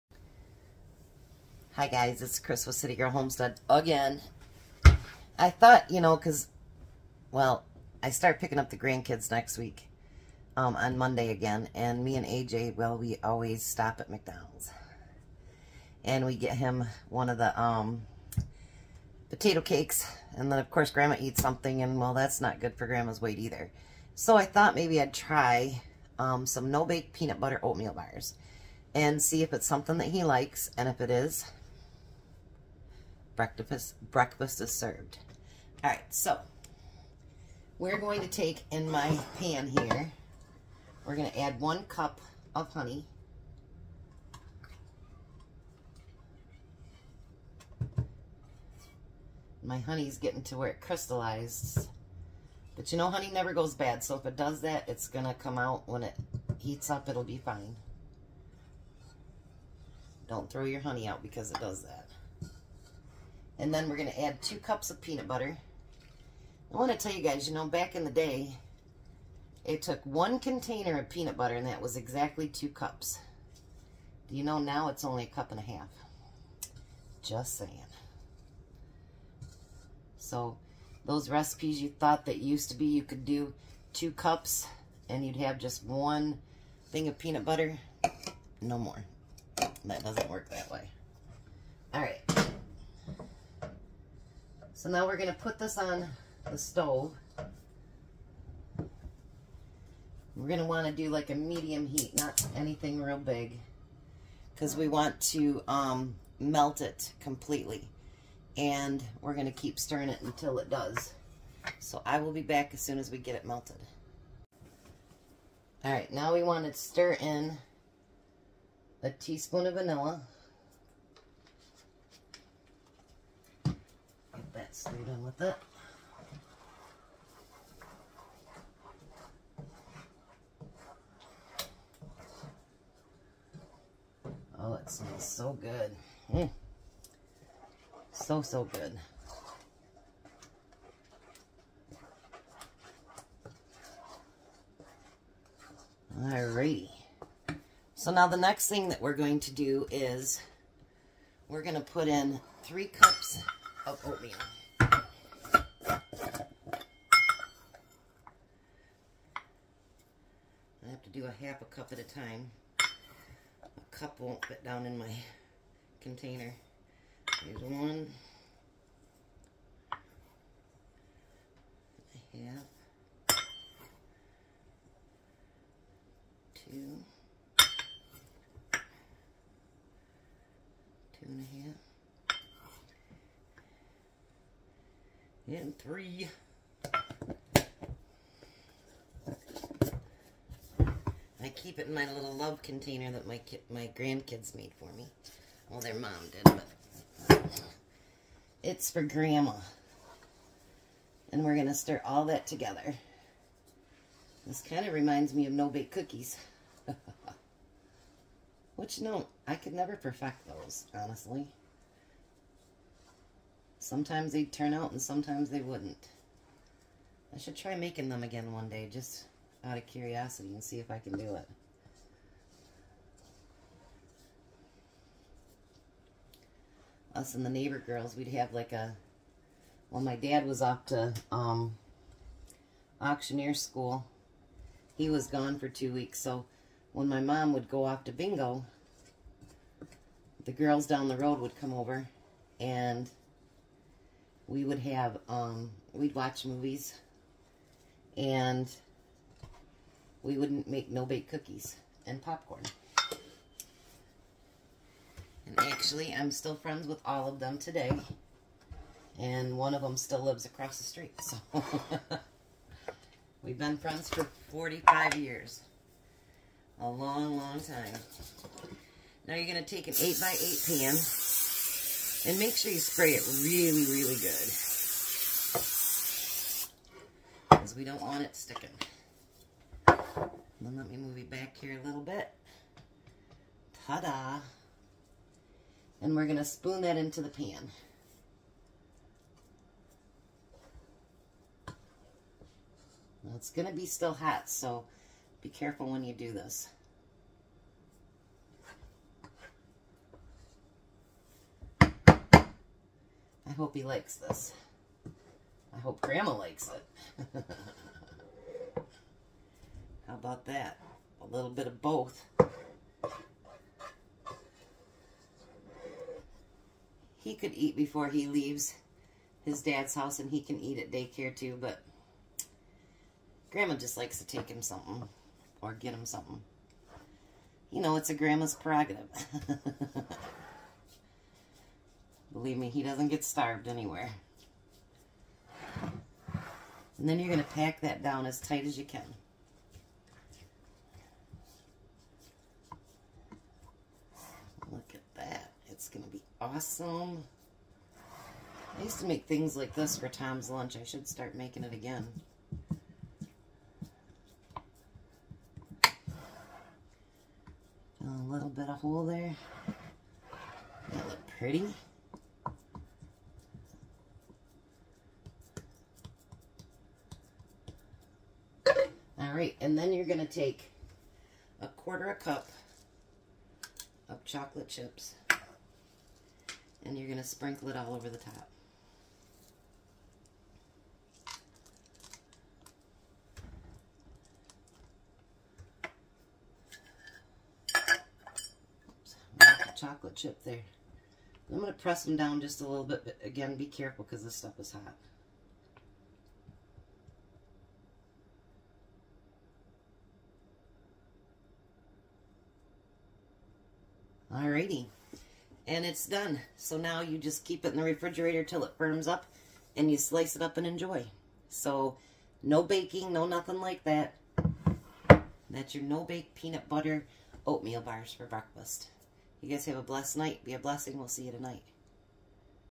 No bake peanut butter oatmeal bars gluten free
2 cups peanut butter
1 cup honey
3 cups old fashioned oats
1 teaspoon vanilla
¼ cup Chocolate chips

In a medium saucepan, mix peanut butter and honey over medium heat until melted (stir constantly).
Stir in vanilla.
Remove from heat. While hot, mix in oatmeal until all ingredients are combined.
Pour into well greased 8x8 baking dish and distribute evenly.
Sprinkle chocolate chips on top
Place in refrigerator until it firms up.